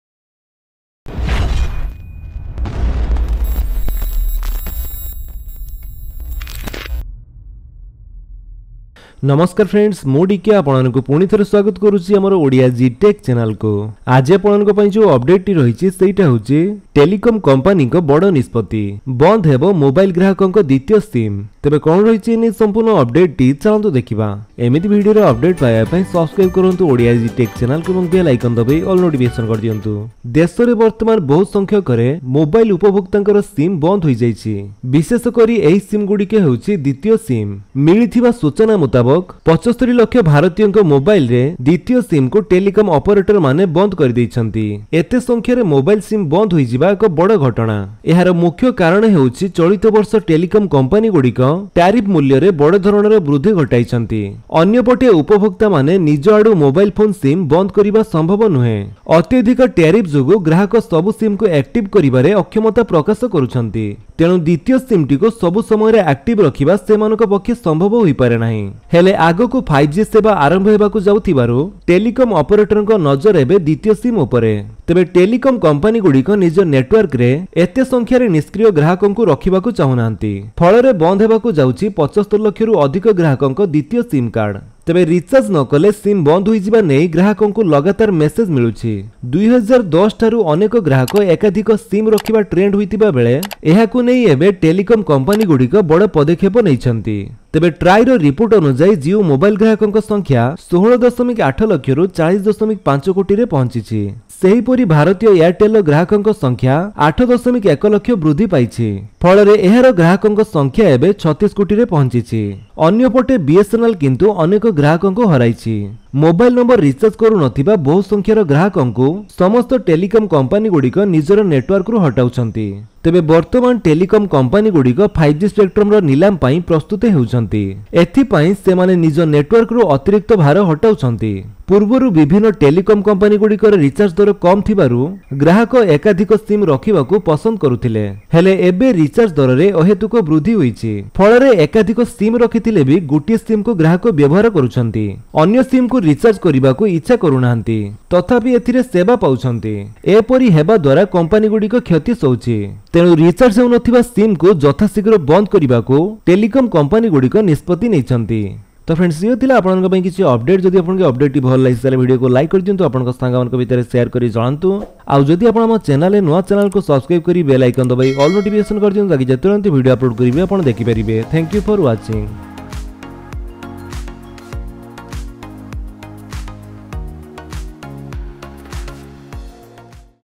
Big Decision Of The Telecom Company | Jio Airtel Second Sim Card Of 75 Lakh Indian Users Was Closed
My 1st Mic Buy Amazon

My 2nd Mic Buy Amazon